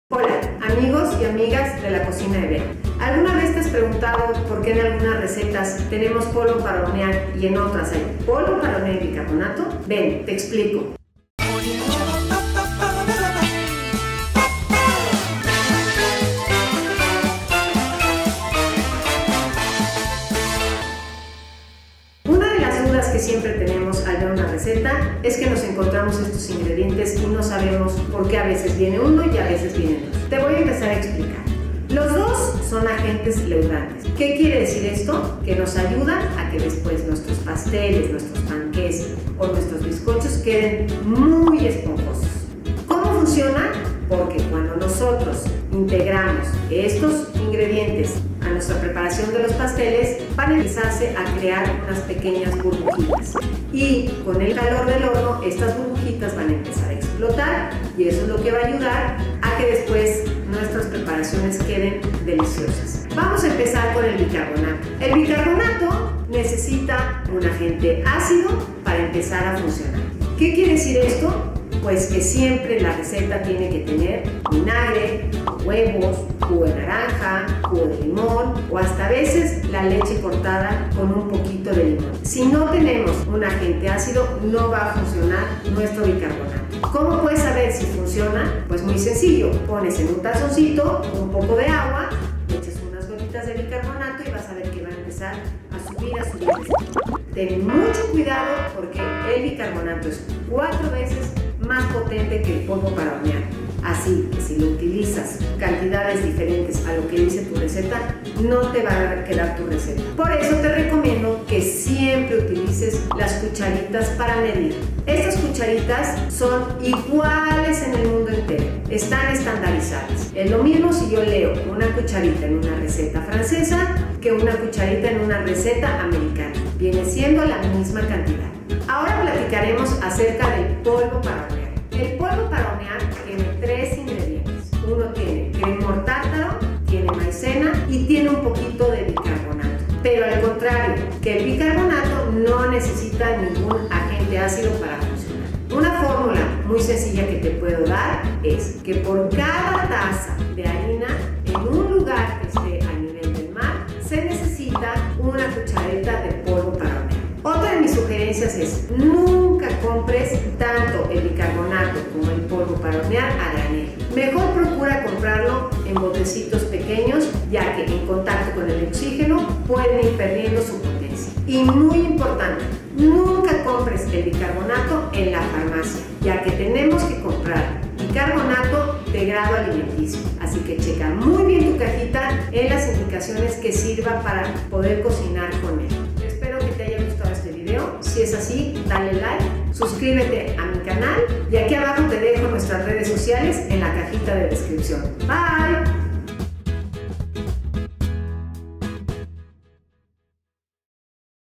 Haz Pasteles Esponjosos SIEMPRE
Esto es muy importante para que tus pasteles queden siempre esponjosos